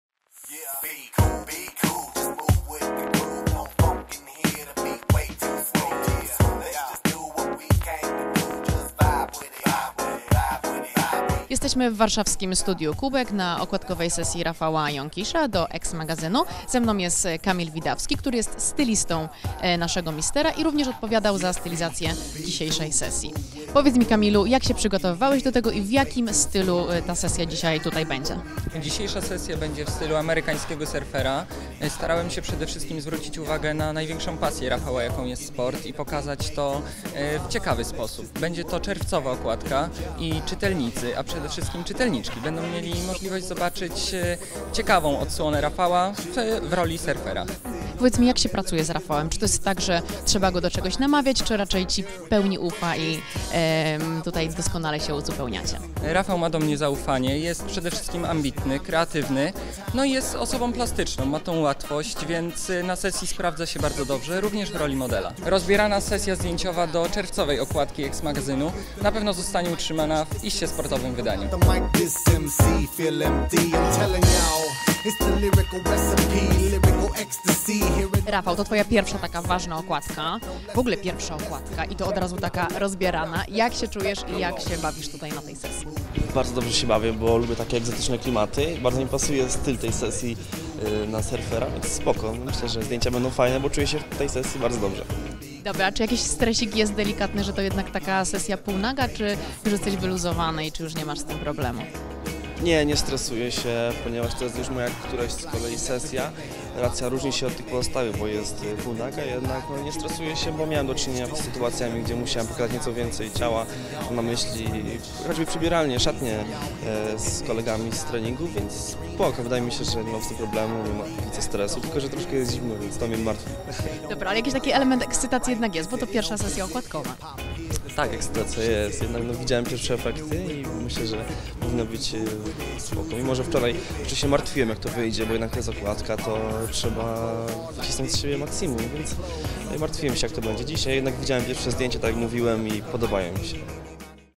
Mister Polski Rafał Jonkisz pokazał ciało dla Eks magazynu!
Od trzech miesięcy Rafał Jonkisz rozwija skrzydła w polskim show-biznesie. Gdy w listopadzie ubiegłego roku osiemnastoletni mieszkaniec Rzeszowa został okrzyknięty najprzystojniejszym mężczyzną w kraju, jego życie obróciło się o 180 stopni. Dzisiaj Rafał ma już miano prawdziwej gwiazdy. Ma na swoim koncie sesje zdjęciowe, a już 4 marca zobaczymy go na parkiecie polsatowskiego show, Taniec z Gwiazdami.